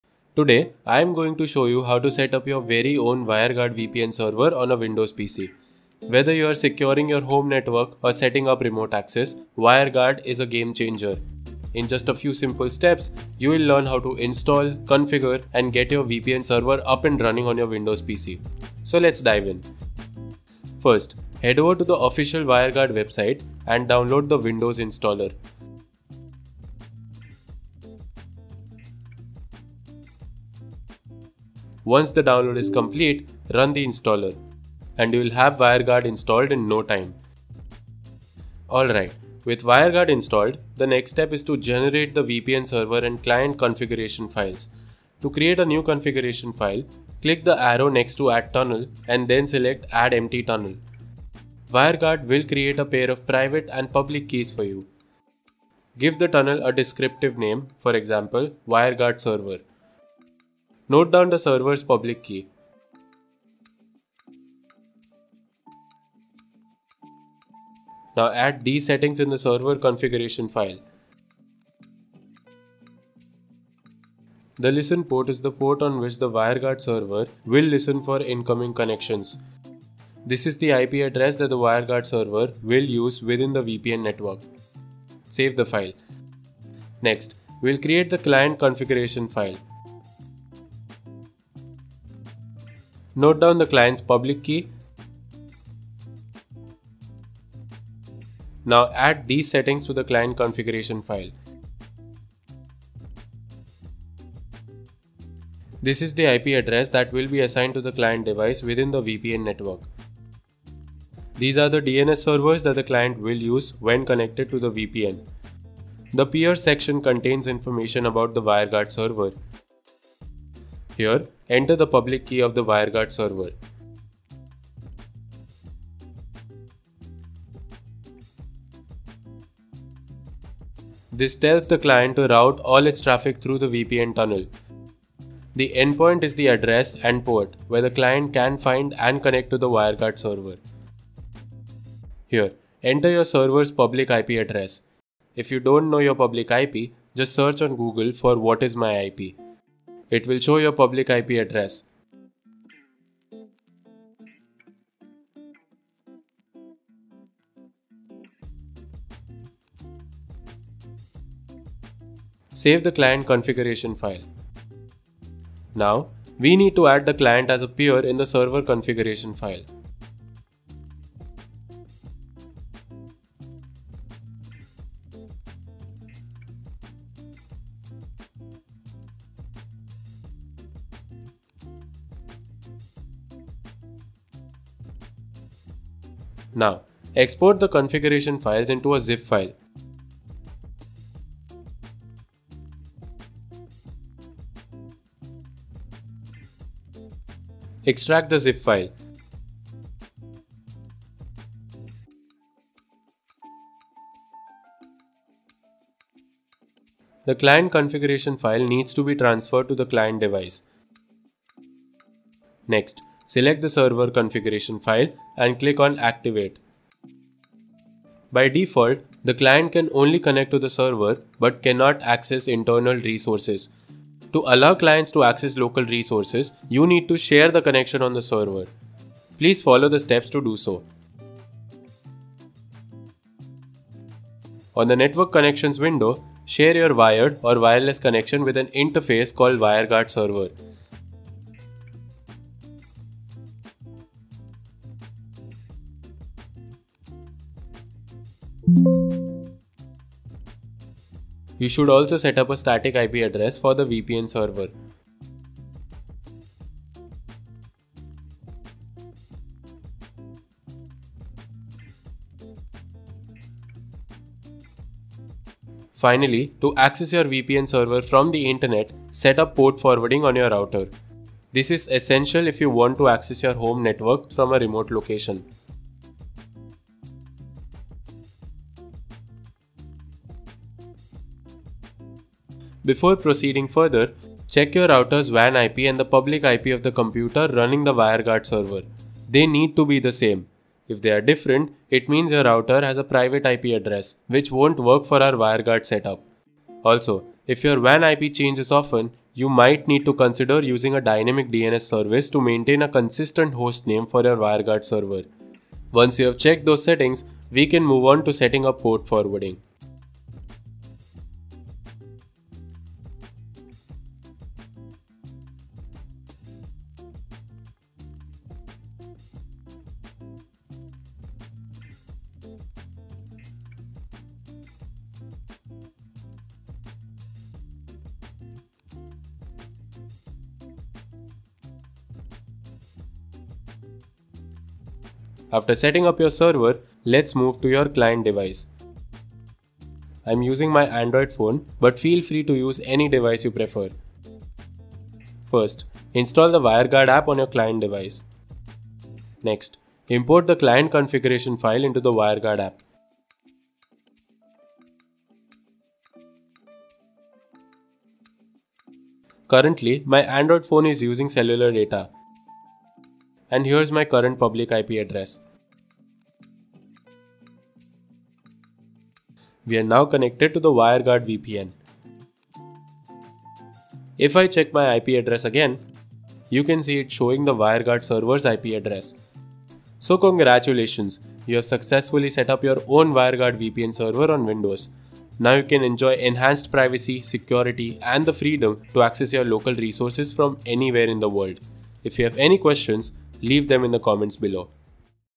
Install and Configure WireGuard VPN Server on Windows PC
In this video, I'll walk you through the entire process of setting up a WireGuard VPN server on a Windows PC - from downloading and installing the software to configuring the server and creating client connections. Whether you're looking to secure your internet connection, access your home network remotely, or ensure your privacy online, WireGuard offers a fast and reliable solution.